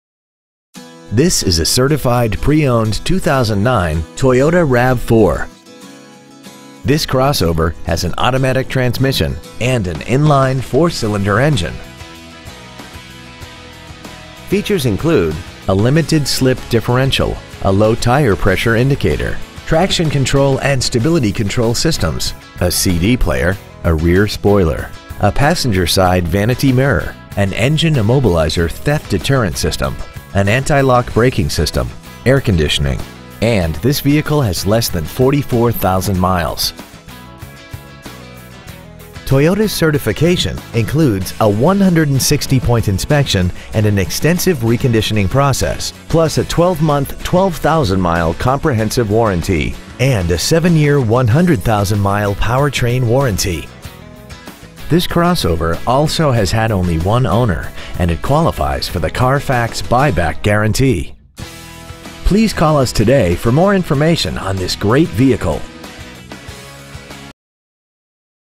2009 Toyota Rav4 Certified San Antonio Texas 78238
Year : 2009
 Make : Toyota
 Model : Rav4
 Engine : Gas I4 2.5L/152
 Trans . : Automatic
 Exterior : Classic Silver Metallic
 Miles : 43,617
 Interior : Gray
 Stock : 22544A

 5730 N.W. Loop 410

 Pre-Owned 2009 Toyota Rav4 San Antonio Texas